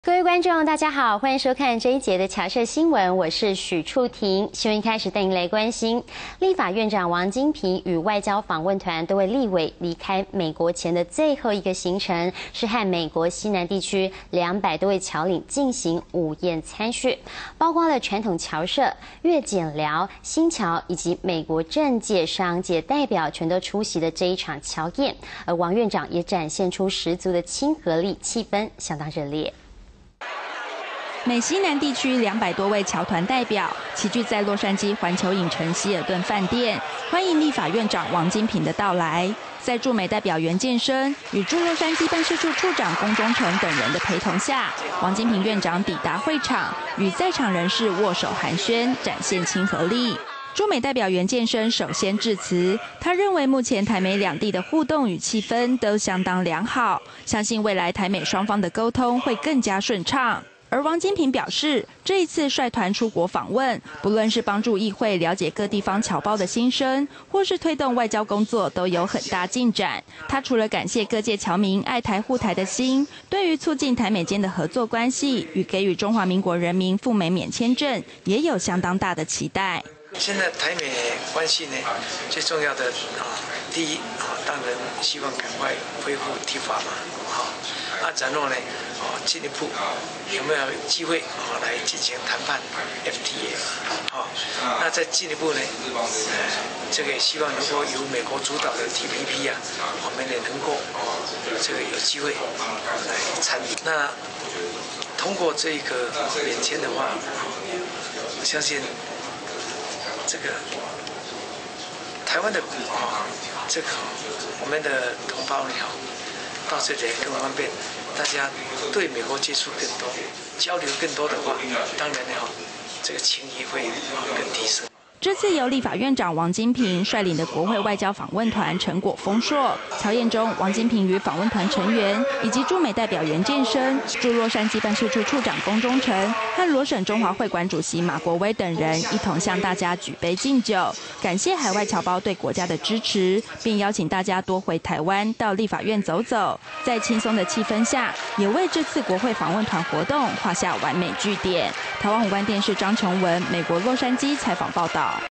立法院長王金平洛杉磯僑宴 - 僑社新聞 09-12-12
美國 洛杉磯
立法院長王金平洛杉磯僑宴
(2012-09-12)
美西南地區兩百多位僑團代表
齊聚在洛杉磯環球影城希爾頓飯店
歡迎立法院長王金平的到來
在駐美代表袁健生
與駐洛杉磯辦事處處長龔中誠
等人的陪同下
王金平院長抵達會場
與在場人士握手寒暄展現親和力
駐美代表袁健生首先致詞
他認為目前美台兩地的
互動與氣氛都相當良好
相信未來台美雙方的溝通
會更加順暢
而王金平表示
這次率團出國訪問
不論是幫助議會
瞭解各地方僑胞的心聲
或是推動外交工作都有很大進展
他除了感謝各界僑民愛台護台的心
對於促進台美間的合作關係
與給予中華民國人民赴美免簽證
也有相當大的期待
現在台美關係最重要的
第一當然希望趕快恢復TIFA
然後呢 進一步有沒有機會
來進行談判FTA
那再進一步呢
這個希望這個由美國主導的TPP
我們也能夠這個有機會來參與
那通過這一個免簽的話
我相信這個台灣的苦
這個我們的同胞呢 到這裡來更方便
大家對美國接觸很多
交流更多的話
當然這個情誼會更提升
這次由立法院長王金平率領的
國會外交訪問團成果豐碩
僑宴中 王金平與訪問團成員
以及駐美代表袁健生
駐洛杉磯辦事處處長龔中誠
和羅省中華會館主席馬國威等人
一同向大家舉杯敬酒
感謝海外僑胞對國家的支持
並邀請大家多回台灣 到立法院走走
在輕鬆的氣氛下
也為這次國會訪問團活動
畫下完美句點
台灣宏觀電視 張瓊文
美國洛杉磯 採訪報導